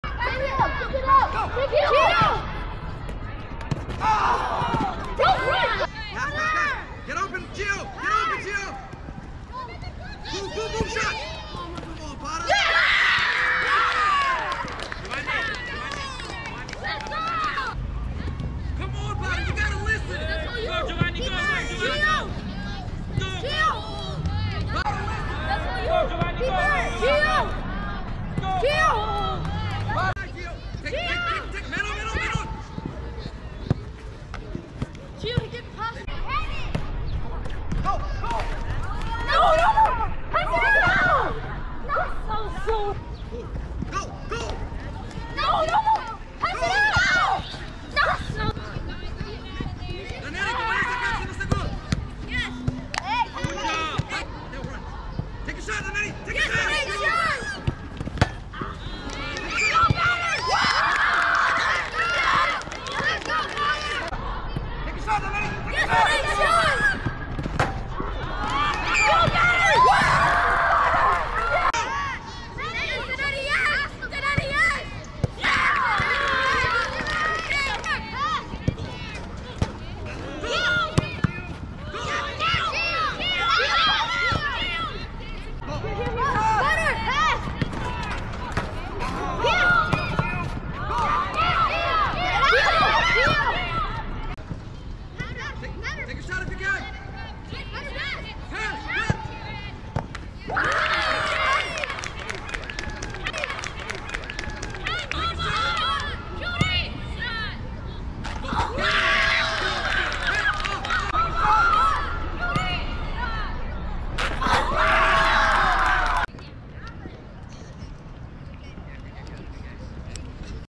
FPYC U12B vs SOTH-R 1 May 5 - HIGHLIGHTS - Score 6:0 - SOCCER U12
5th Game of the 2024 Spring Season
HIGHLIGHTS of Coach Jay's FPYC Vs DC on May 5th 2024
FPYC won 6:0 with goals from
3x Badr
1x Levi
1x Mohad
1x Daneri